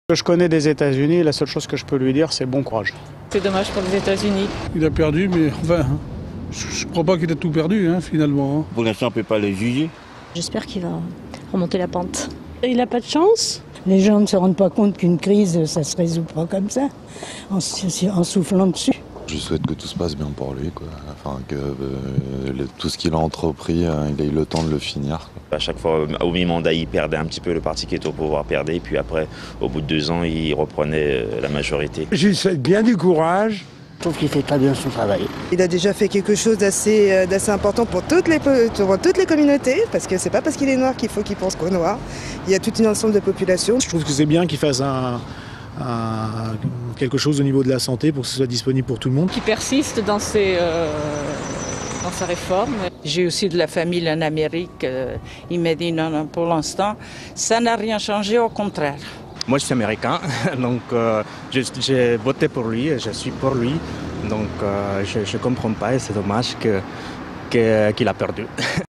Obama toujours dans le coeur des français (Evry)
Les essonniens expriment leur soutien envers le président des États Unis. (DROITS RESERVES . Pour toute exploitation sur TV, web, mobile, aller